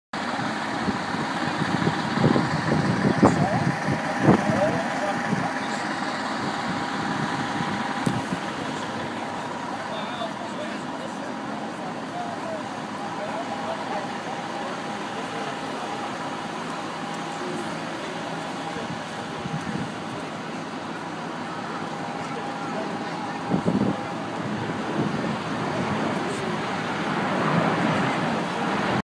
KCSO video of aftermath of crash in Burien Monday, May 13, 2013
King County Sheriff's Office video of the aftermath of a mulit-vehicle crash in Burien on May 13, 2013.